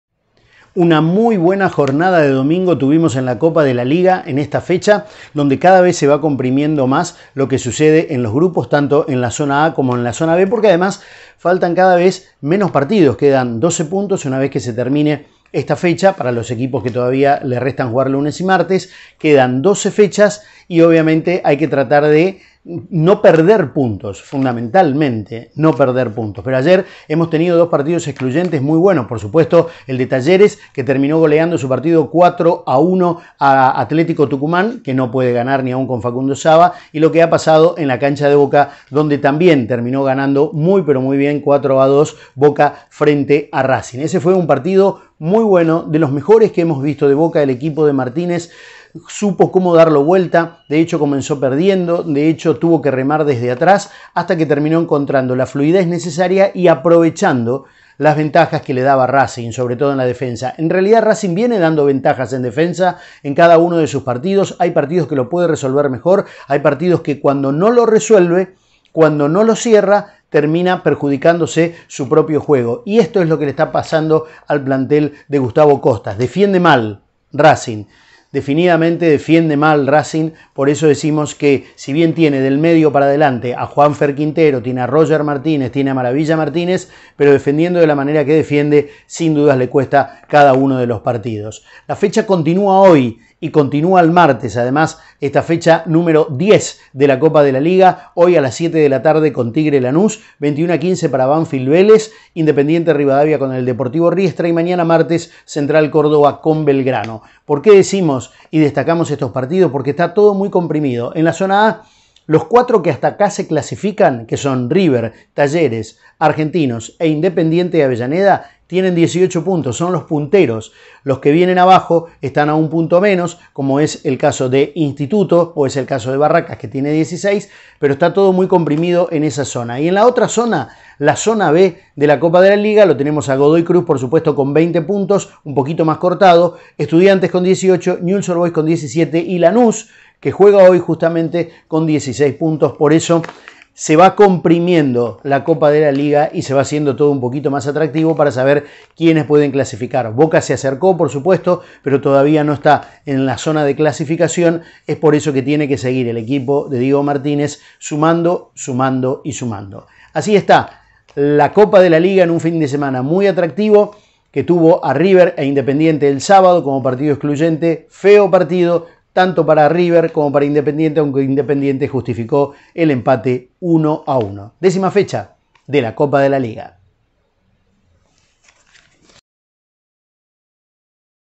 Boca-Racing y Talleres-Atlético Tucuman redondearon un gran domingo en la Liga
Video de Coco Ramón